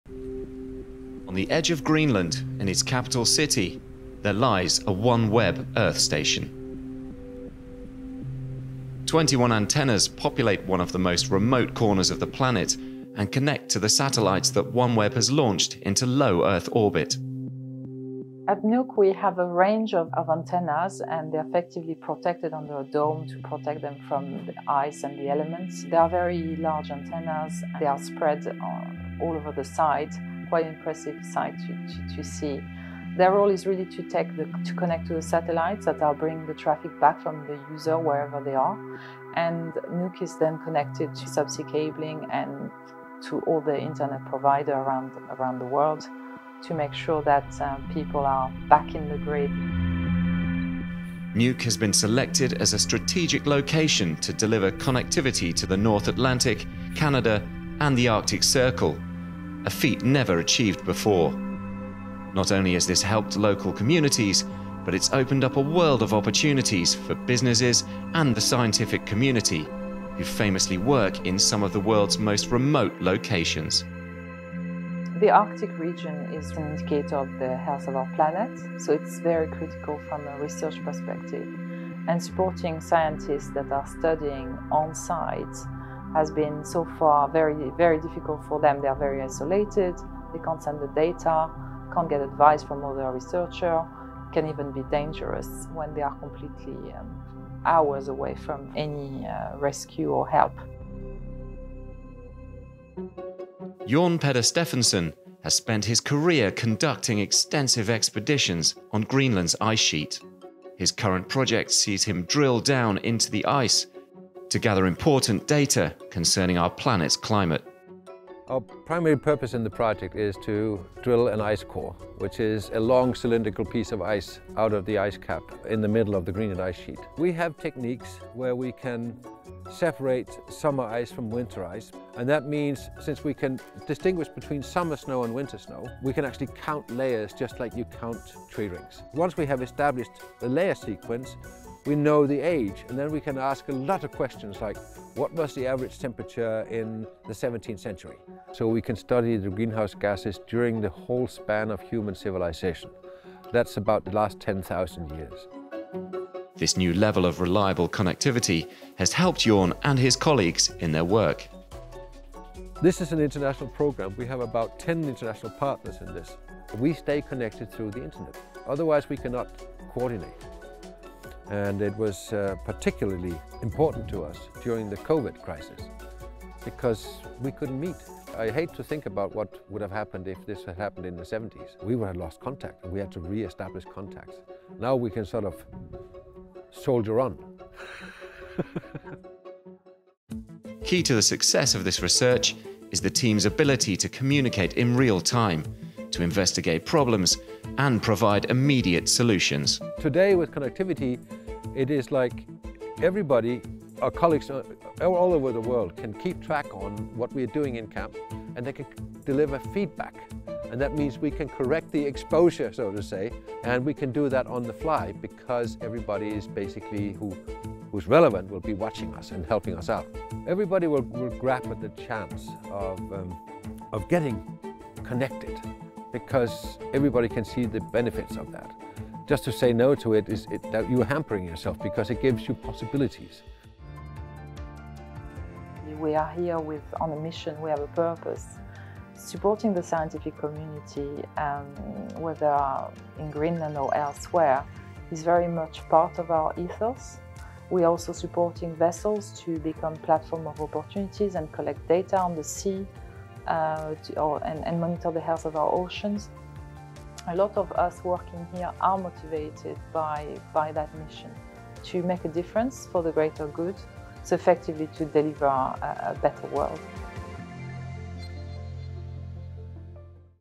OneWeb - Connectivity to serve research and climate change studies
Low Earth orbit network solutions like OneWeb have the unique ability to serve high-speed, low-latency connectivity to locations never connected before, including science and research organisations who are deepening our understanding of climate change in remote places like Greenland.